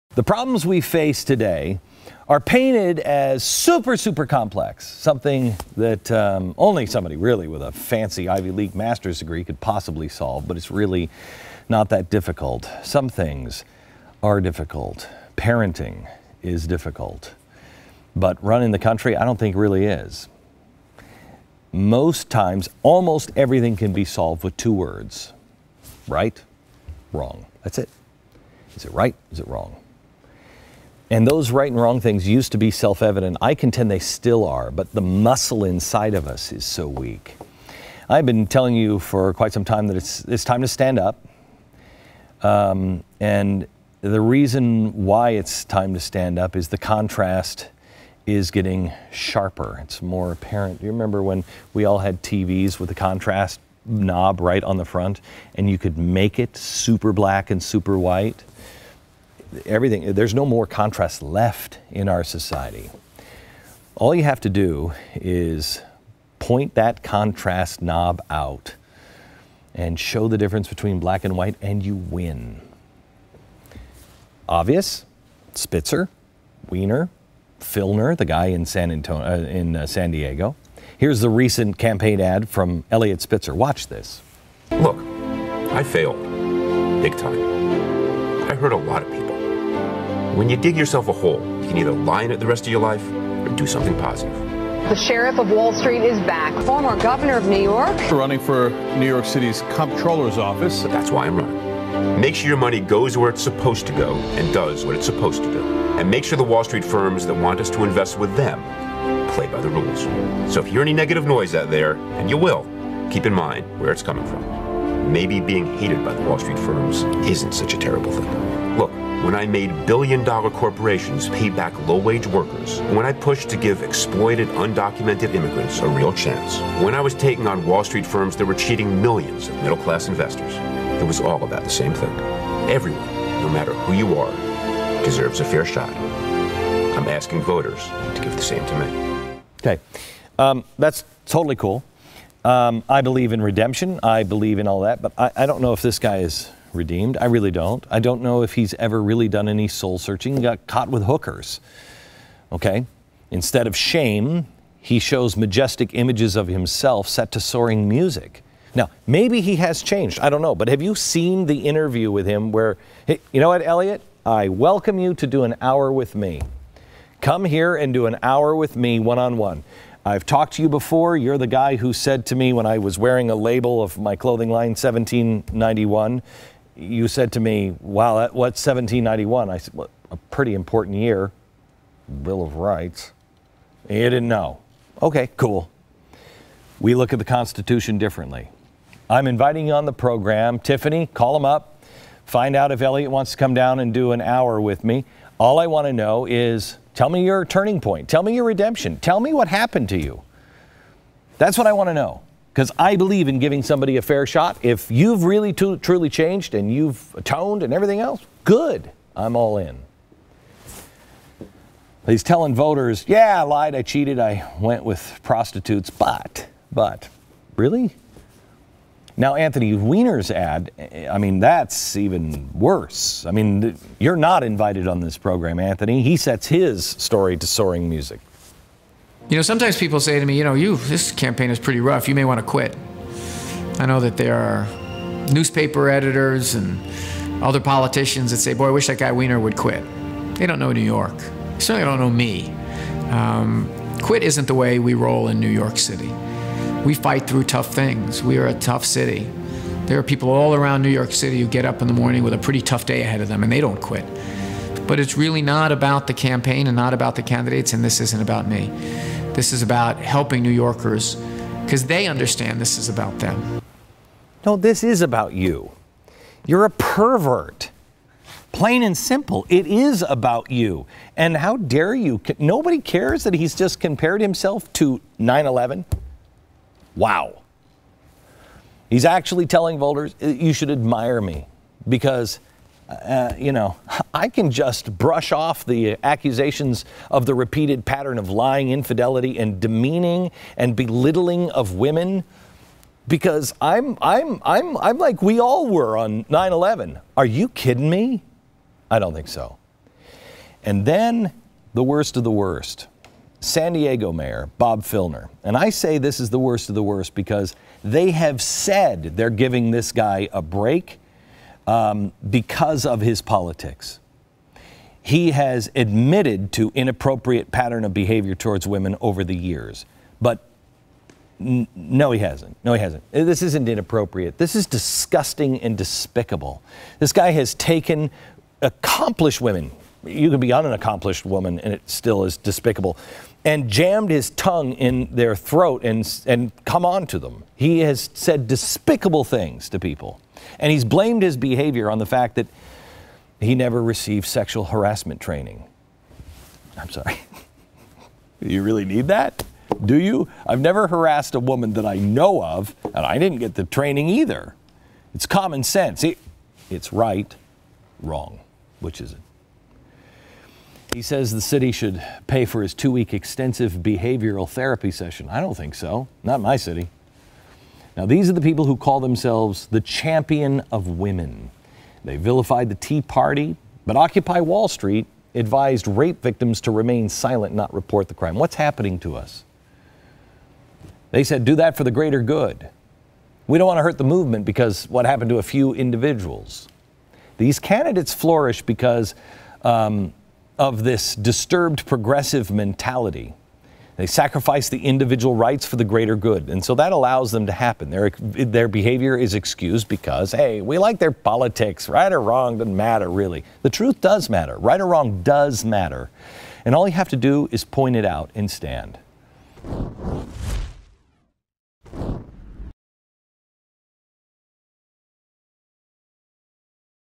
Schneiderman Scandal is Nothing New | Looking Back (2013)
Glenn's monologue from 2013 shows we were having the same problems as we were in the past. Elliot Spitzer, Anthony Weiner, and Bob Filmer were up to the same shenanigans but were treated differently than the politicians today.